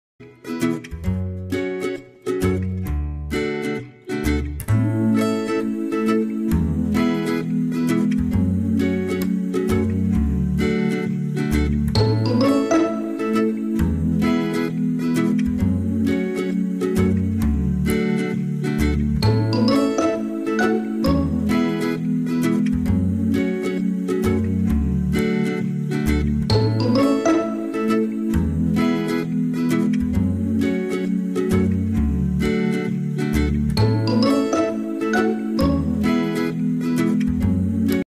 Throwback at our October Science Camps - Mad Science London
Throwback at our October Science Camps when we did the awesome experiment of dry ice bucket! Kids had a great time and so did we. Everyone has fun with Mad Science!